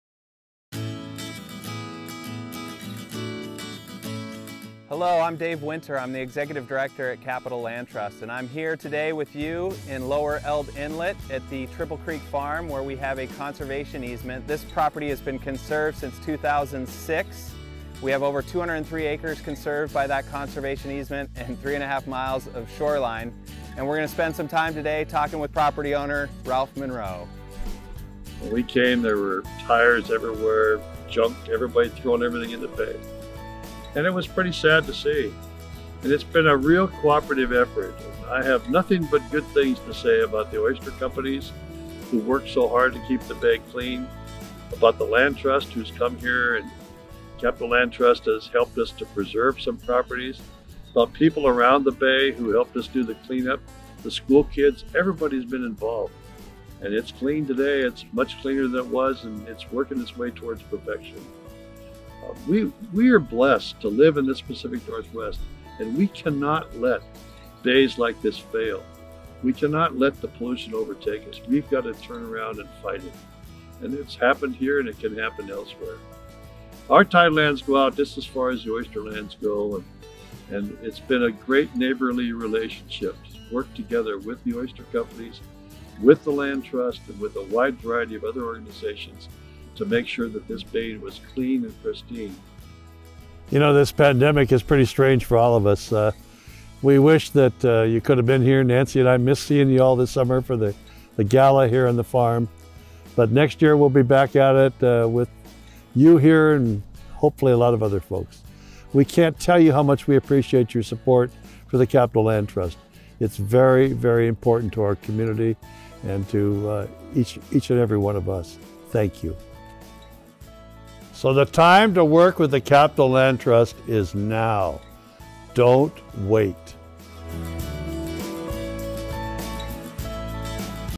A Message From Ralph Munro
Triple Creek Farm is the cornerstone of Capitol Land Trust’s lower Eld Inlet habitat protection plan that encompasses approximately 450 acres and 3.5 miles of marine shoreline. Triple Creek Farm can be admired from the William Cannon Footpath across the mud flats, which the Munros played a major role in creating. Regular inhabitants of Triple Creek Farm include bald eagles, red-tailed hawks, great blue heron, deer, coyote, cougar, and five species of anadromous fish.

To find out how you can support local land conversation
Drone Footage: Mike Melton